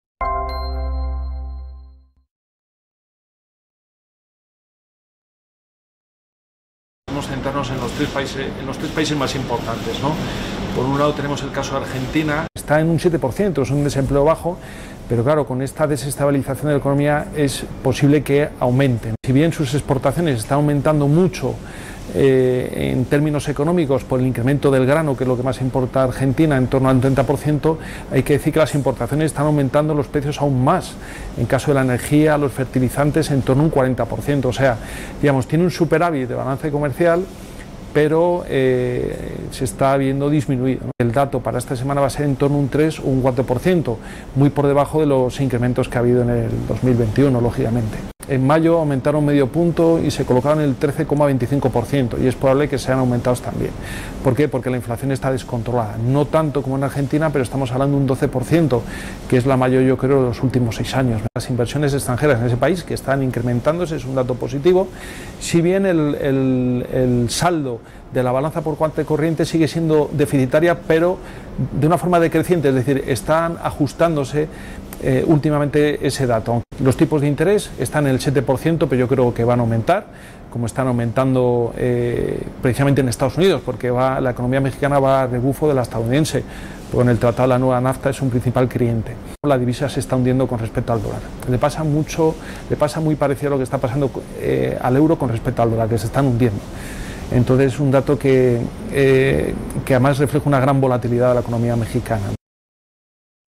Economía se proyecta así en países de América Latina, esta semana
Argentina publica esta semana la evolución del PIB del primer trimestre del año, tras el tras el repunte del 10,3 % que registró la economía en 2021 tras tres años en recesión, y los datos del paro, que a fines del año pasado afectaba al 7 % de la población.